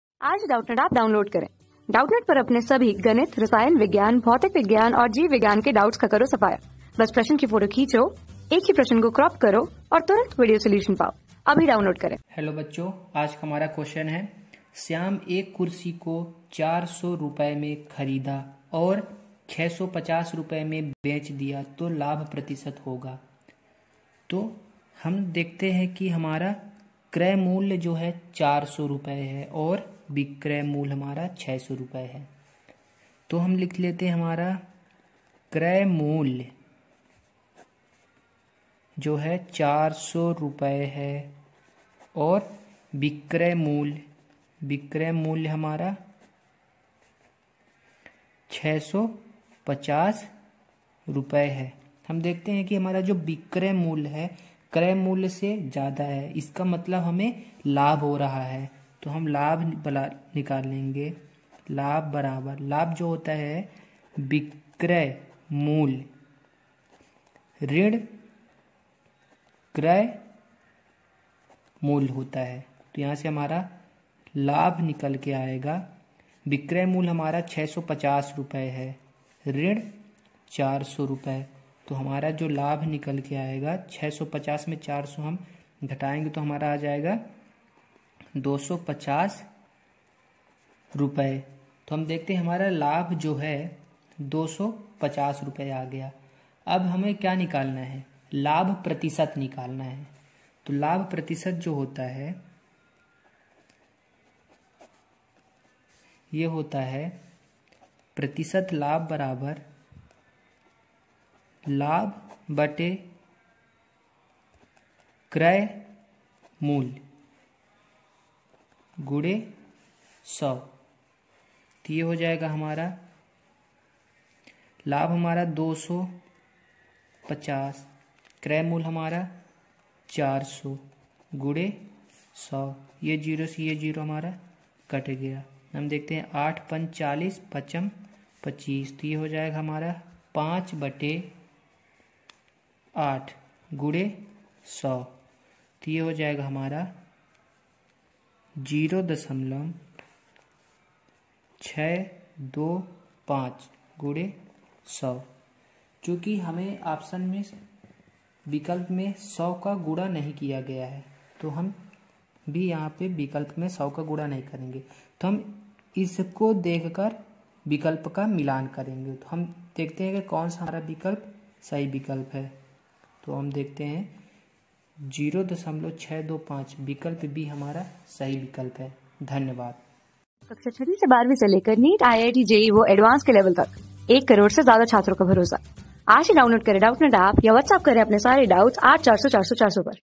श्याम, एक कुर्सी को 400 रुपये में खरीदा और 650 रुप बेच दिया, तो लाभ प्रतिशत होगा | 12 | उ.प्र सय...
श्याम, एक कुर्सी को 400 रुपये में खरीदा और 650 रुप बेच दिया, तो लाभ प्रतिशत होगा
Class: 12
Subject: MATHS
Chapter: उ.प्र सयुक्त प्रवेश परीक्षा पॉलिटेक्निक, 2005
Board:Polytechnic

👉 Have Any Query? Ask Us.